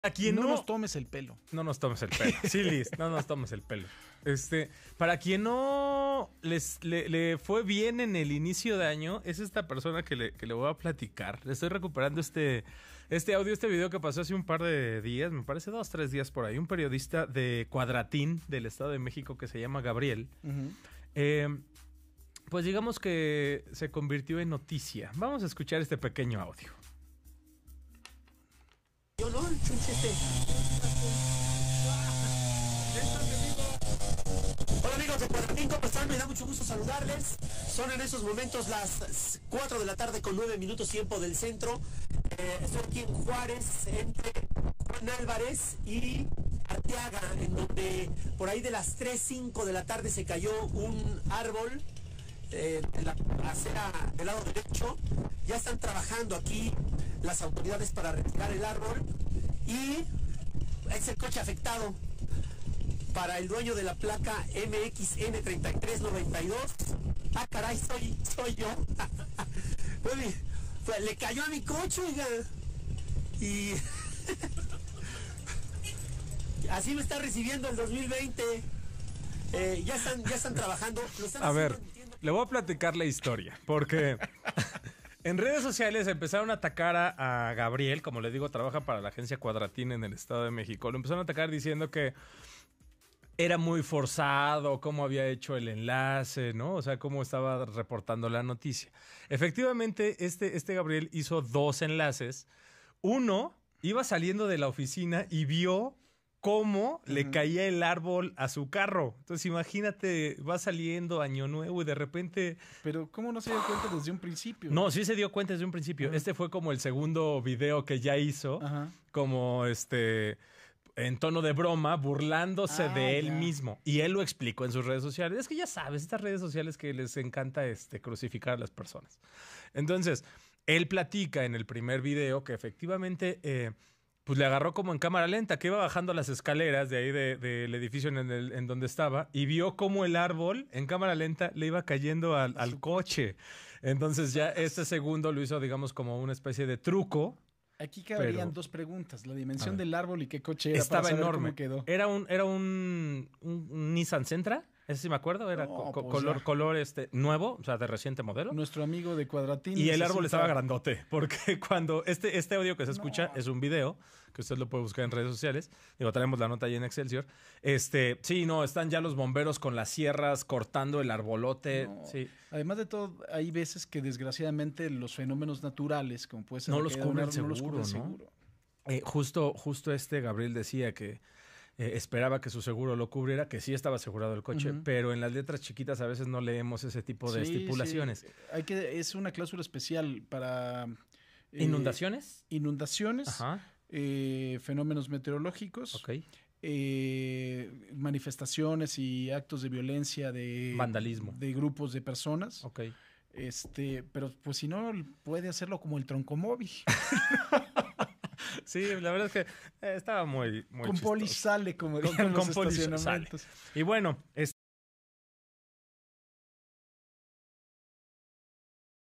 Reportero transmite caída de árbol sobre un auto... era el suyo
El periodista se hizo viral por su reacción al informar que era su vehículo el que había resultado dañado tras la caída de un árbol, por los fuertes vientos.